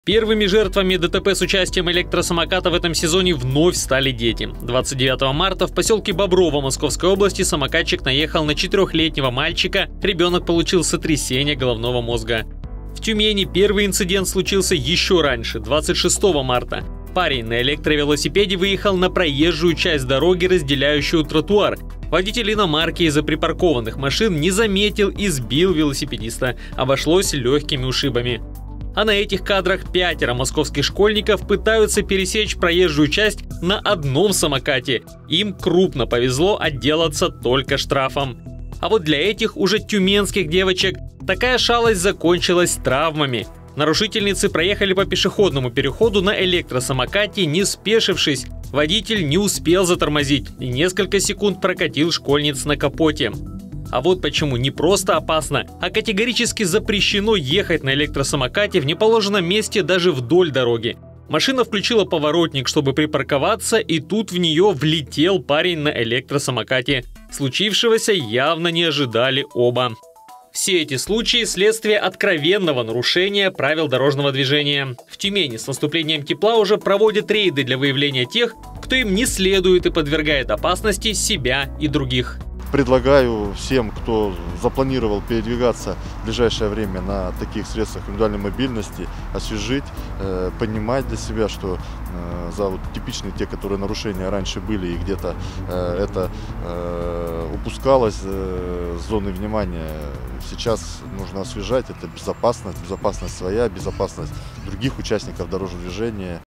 Тюмень Самокатная
Водителей самокатов и велосипедов уже начали штрафовать. И не зря - в прошлом году в области произошло 194 ДТП со средствами индивидуальной мобильности. Тысячи таких аварий происходят каждый год по всей стране. Об опасностях этого популярного транспорта - в нашем сюжете.